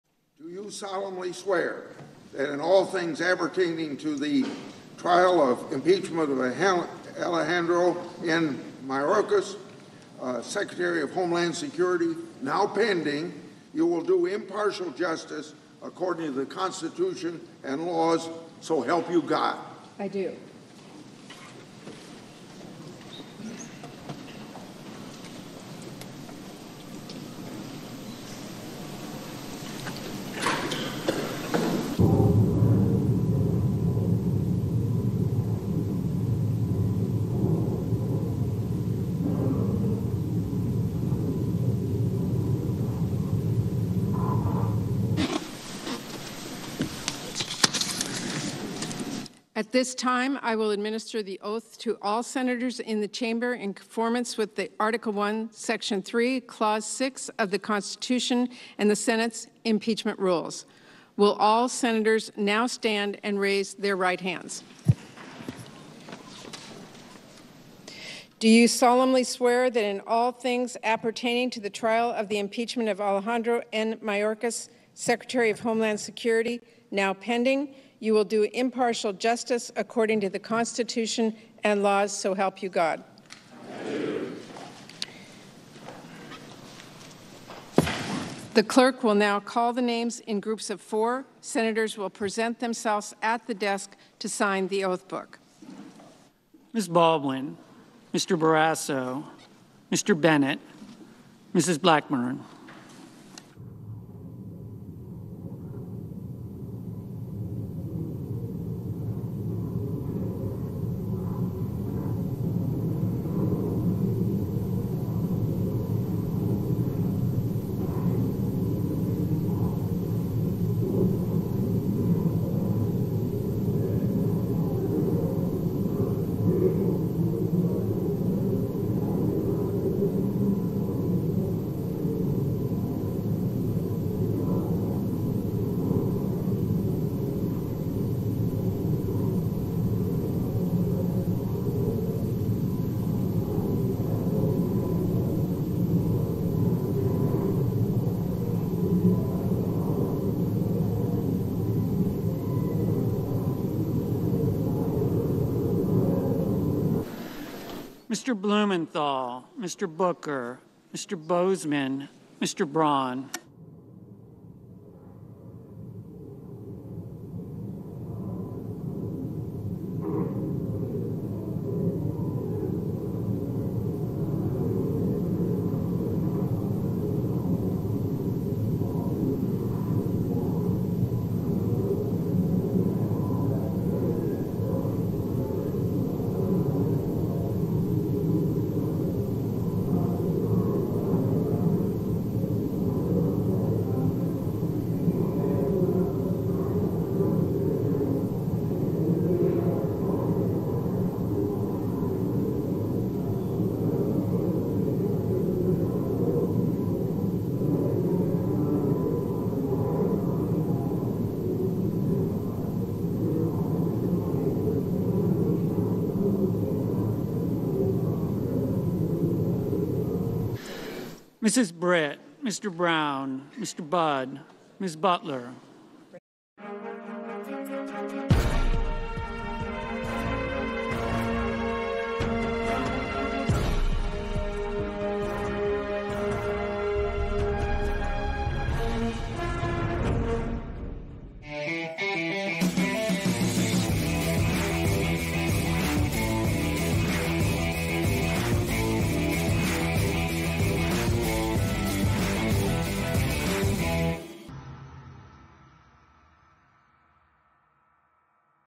BREAKING: U.S. Senate sworn-in for Mayorkas trial
The United States Senate is sworn-in for the impeachment trial of Alejandro Mayorkas.